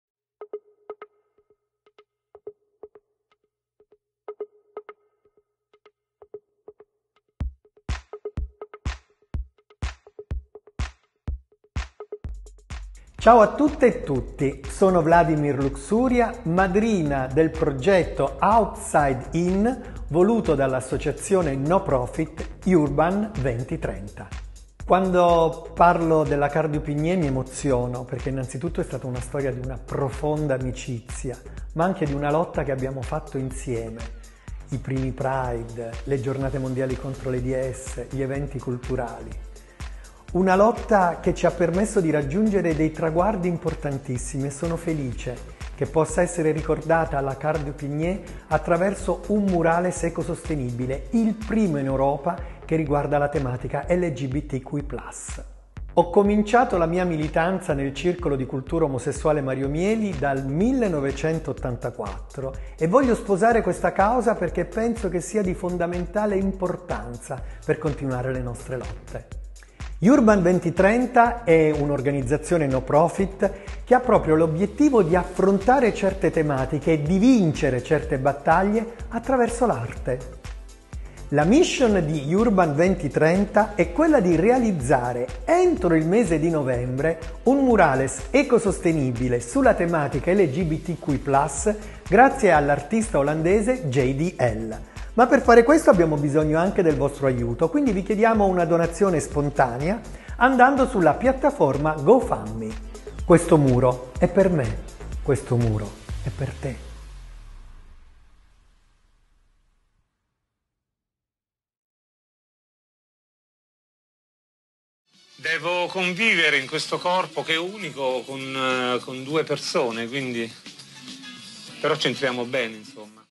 OUTSIDE IN : GREEN MURALES BY JDL STREET ART
No wall breaks down by itself and the walls of prejudice are the most difficult to tear down. After Hunting Pollution, the largest Europe largest green mural , Yourban2030 will  create the first green mural on the theme of gender identity. Yourban2030  wants to do it together with you, making you the protagonists of this new adventure.

Each donor will see their name on a plaque that will be affixed to the base of the mural, to thank all those who strongly believe, like us, in the universal power of art and its communicative power.

A wall for everyone and of everyone, a green wall that acts against pollution through  the use of special paints that neutralize pollutants . A wall that wants to break down the many barriers  of prejudice, a work of art that narrates the dialogue with one's gender identity and on the right of  freedom of expression of one’s self .

The mural is dedicated to Karl Du Pignè (Andrea Berardicurti), icon of the LGBTQ + world, passionate and tireless activist of the Mario Mieli social club and Drag Queen who died aged only 61 in 2018.

Judith de Leeuw, aka JDL street art, is the Dutch artist chosen for this artistic operation who enthusiastically accepted the challenge and who made herself available to create her very first green work in the capital.

To give strength to this project, we have an exceptional Ambassador , Vladimir Luxuria, who with Karl du Pignè shared an important piece of road for the claim of the rights of LGBTQ + movement .

The first green mural dedicated to the LGBTQ + movement will be created in collaboration with the Mario Mieli homosexual culture club, the Dutch Embassy in Italy and with the patronage of the VIII Municipality of Rome.

Nessun muro si abbatte da solo e i muri del pregiudizio sono quelli più difficili da abbattere.

Dopo Hunting Pollution, il murales green più grande d'Europa, Yourban2030 vuole realizzare  il primo murales green sul tema dell'identità di genere e lo vuole fare insieme a voi, rendendovi protagonisti di questa nuova avventura.

Ogni donatore vedrà il proprio nome riportato su una targa che verrà apposta alla base del murales, per ringraziare tutti coloro che credono fortemente, come noi,  nel potere universale dell'arte e nella sua forza comunicativa.

Un muro per tutti e di tutti,  un muro green perché agisce contro l’inquinamento attraverso l'utilizzo di speciali pitture che neutralizzano agenti inquinanti ma anche un muro che vuole abbattere i tanti muri del pregiudizio,  un'opera d'arte che racconta il dialogo con la propria identità di genere e che invita alla riflessione sulla libertà di espressione del proprio sè.

Il murales  è dedicato alla Karl Du Pignè (Andrea Berardicurti), icona del mondo LGBTQ+, attivista appassionata e instancabile del circolo Mario Mieli e Drag Queen morta a soli 61 anni nel 2018.
Judith de Leeuw, aka JDL street art, è l'artista olandese scelta per questa operazione artistica che ha accolto con entusiasmo la sfida  e che si è resa disponibile a realizzare la sua primissima opera green nella Capitale.

Per dare forza a questo progetto, abbiamo  una madrina d'eccezione, Vladimir Luxuria, che con la Karl du Pignè ha condiviso un pezzo importante di strada per la rivendicazione dei diritti delle persone LGBTQ+.

Il primo murales green murales dedicato al movimento LGBTQ+ nasce in collaborazione con Il Circolo di cultura omosessuale Mario Mieli,  l'Ambasciata olandese in Italia, con il patrocinio del Municipio VIII di Roma in media partnership con Flewid Book.

ENGLISH